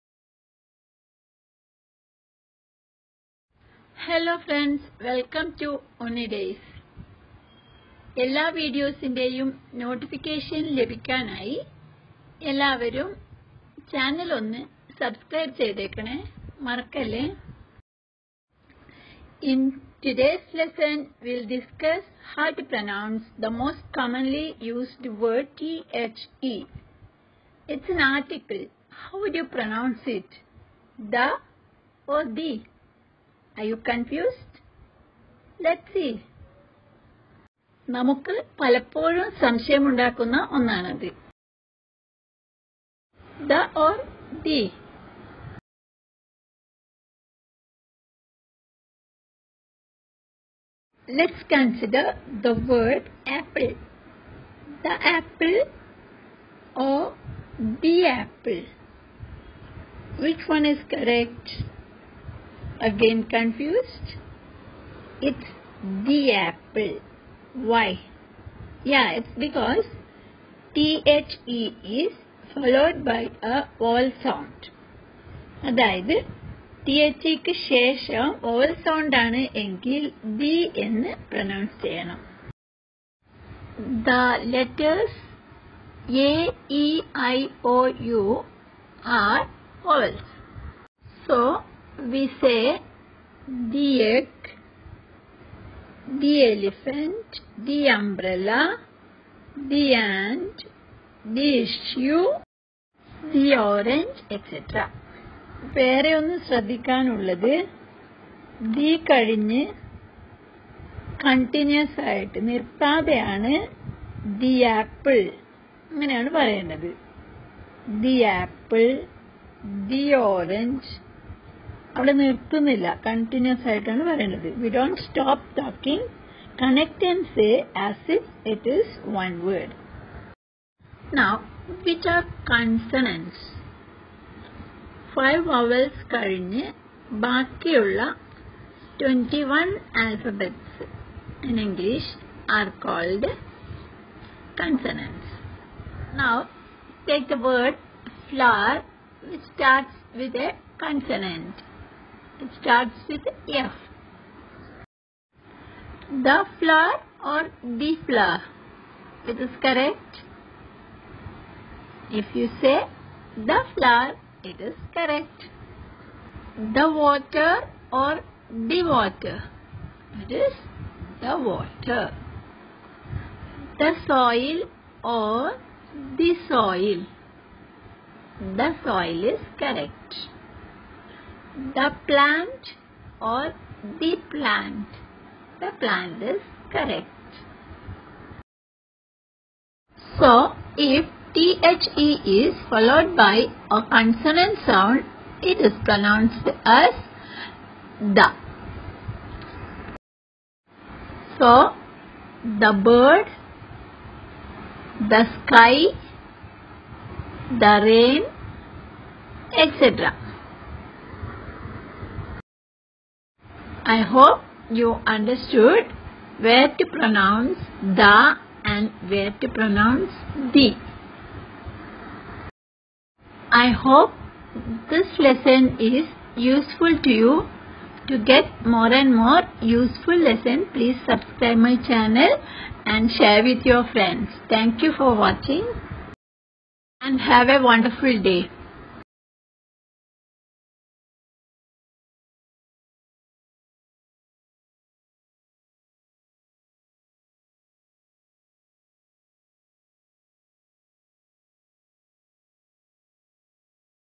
How to pronounce' THE',ദ ( th-uh) orദി( th-ee),How to speak English fluently, Fluent spoken English
How to pronounce  ' THE' .th-ee ദി or th-uh ദ